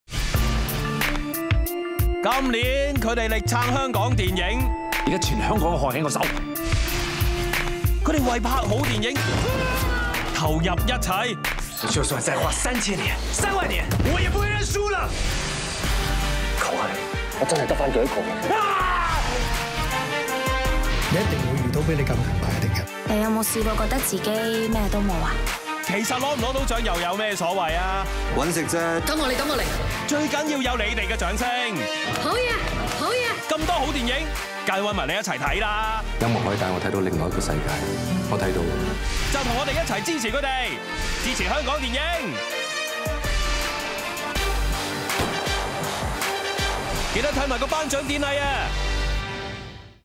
最後倒數-第37屆香港電影金像獎
古仔拎影帝？定係鄭中基？娜姐同Stephy同時首度問鼎，邊個爭得掂？
未知結果前先煲提名電影是常識吧！多套今屆香港電影金像獎提名電影，Now TV畀你逐套睇！星期日再睇埋頒獎禮直播，香港電影一齊撐！

《第37屆香港電影金像獎》
4月15日晚上8點 衛視電影台（頻道139）現場直播

PS:衛視電影台 SCM FAN CLUB 當晚仲會直撃後台，訪問各大贏家得獎感受，金像獎前前後後同你睇到盡！！